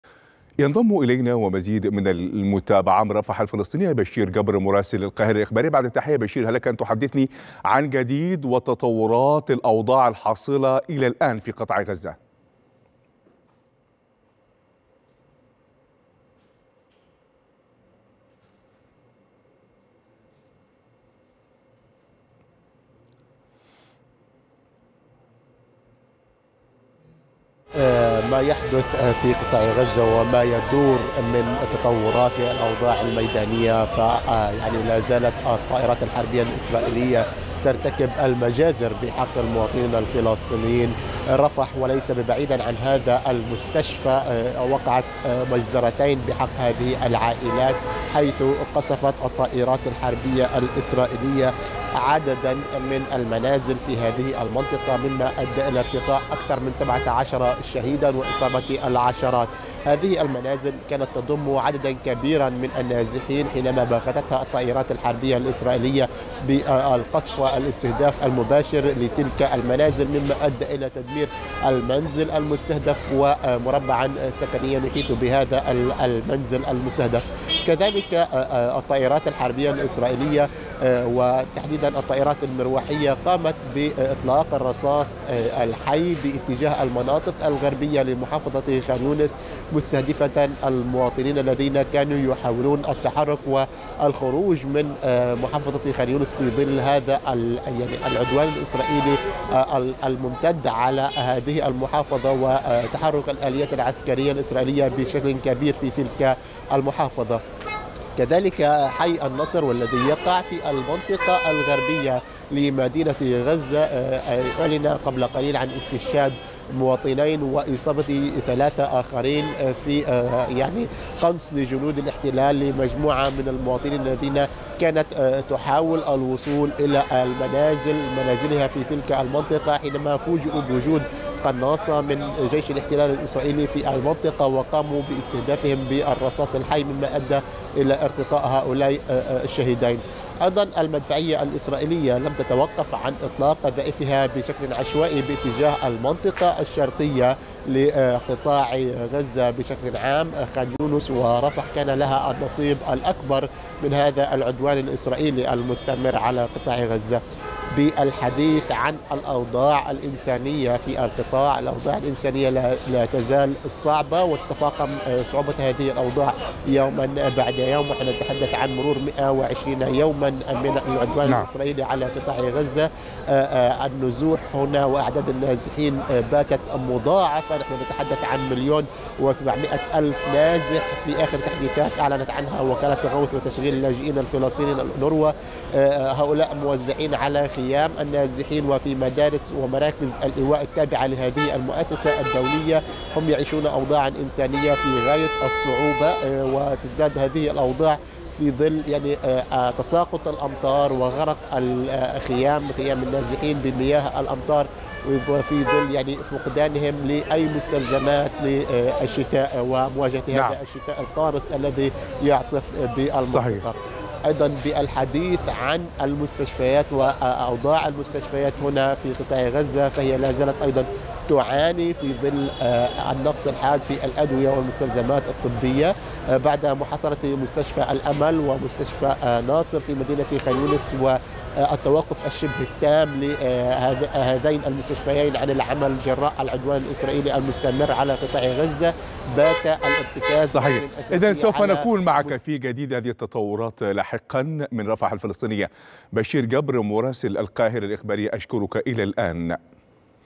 طائرات الاحـ تلال تستهدف المدنيين.. قصف إسرائيلي لعدة منازل في غزة
أكد بشير جبر مراسل القاهرة الإخبارية برفح الفلسطينية، أن طائرات الاحتلال مستمرة في تنفيذ غارات على عدة مناطق بقطاع غزة، والمدفعية الإسرائيلية تطلق القذائق بشكل عشوائي

3/2/2024